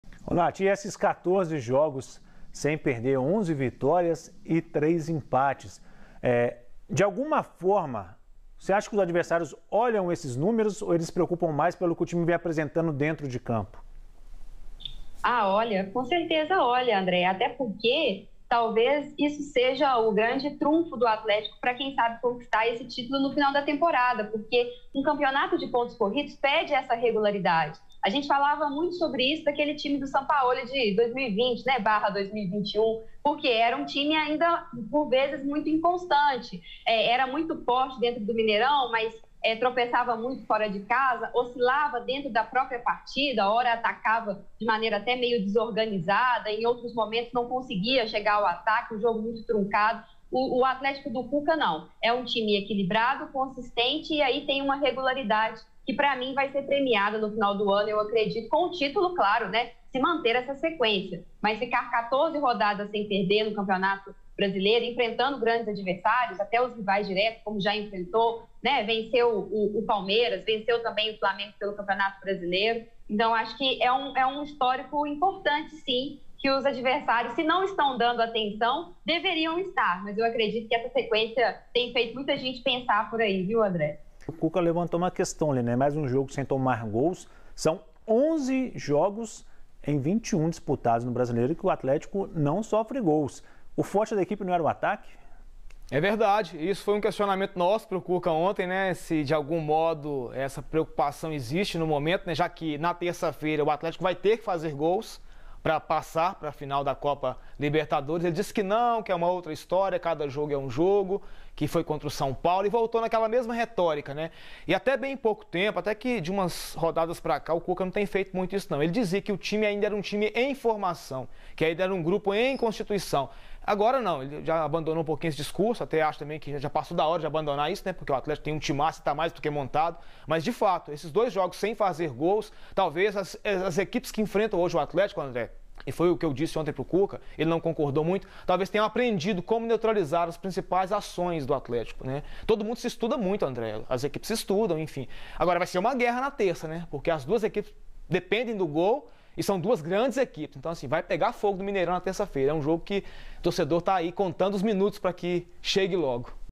ATLÉTICO ESTÁ HÁ 14 JOGOS SEM PERDER! E CONTANDO...
Não é a toa que o Atlético é o líder do Campeonato Brasileiro. Já são 14 jogos sem derrota na competição. Foram 11 vitórias e três empates. E a sequência de invencibilidade não parece estar perto de acabar.